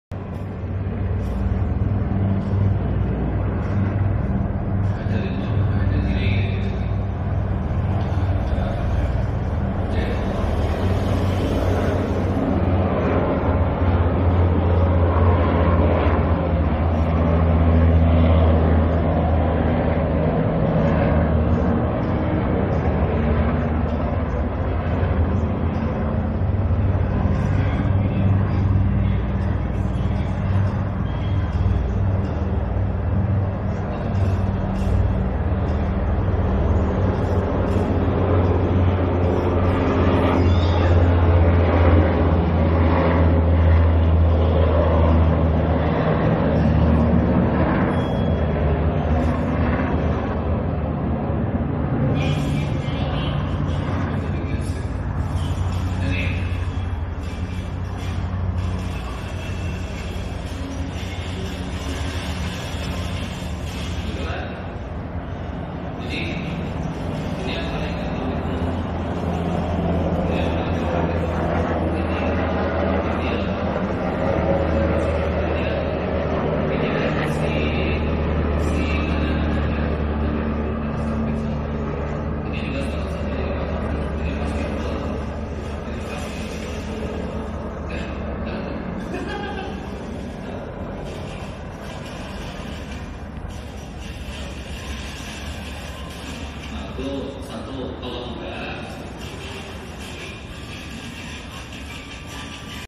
바이든 방문 다음날 지나가는 UH-60 블랙호크 행렬
Comic world 2022 in seoul

i guess They're move to OSAN 7th AFB. when done it mission of Joe Biden Security.

평택.